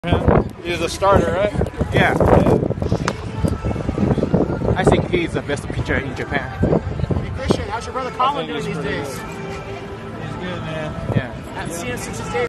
【MVP】イェリッチ 菅野智之を語る Christian Yelich mentioned about Japanese best pitcher Tomoyuki Sugano.
WBC米国代表のイェリッチ選手に、当時対戦した巨人の菅野智之のことを覚えているか？と聞きました。

僕「あなたは日本で有名ですよ」
イエリッチ「イチローとチームメイトだったからだろ？」

ここから動画内の会話
僕「WBCで戦ったスガノ覚えている？」
イエリッチ「もちろん。先発投手だろ？」
僕「僕は彼が日本最高の投手だと思う」
イエリッチ「超グッド投手だと思う。良いよな」
僕「yeah...」